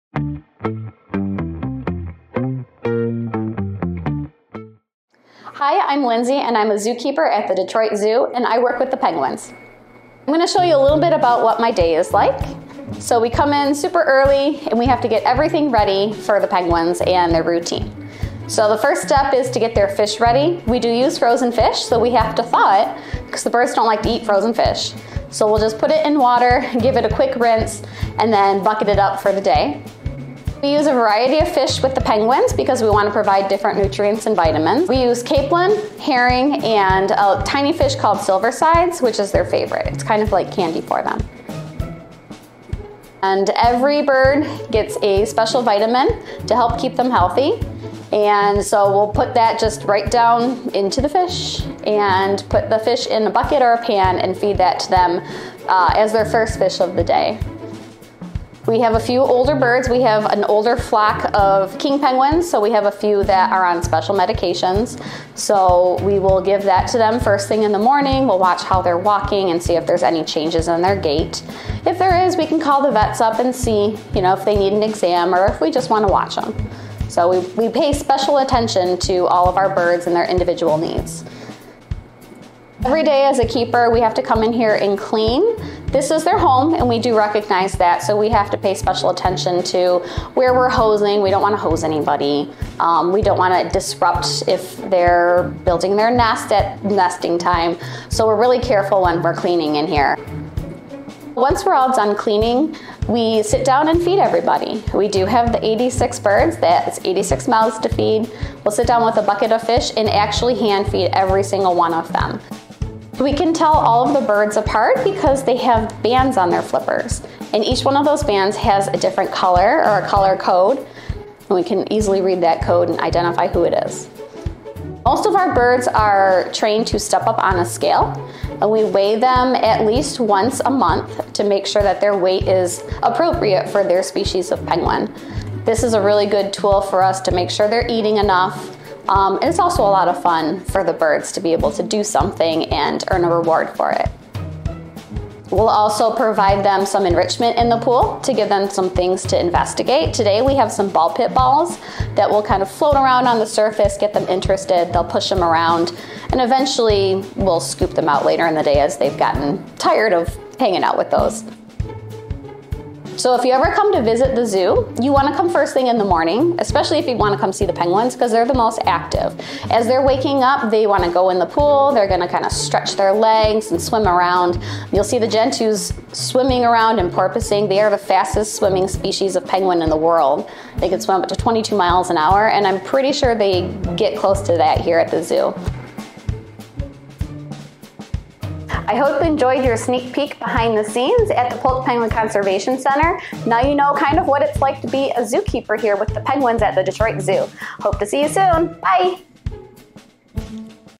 Detroit Zoo – Be a Penguin Zookeeper for a Day!
Be a zookeeper for a day! 🐧

Caring for a waddle of 86 penguins is no small “feet.” Join penguin zookeeper Lindsay before the sun is up for a sneak "beak" behind the scenes at the Detroit Zoo’s Polk Penguin Conservation Center.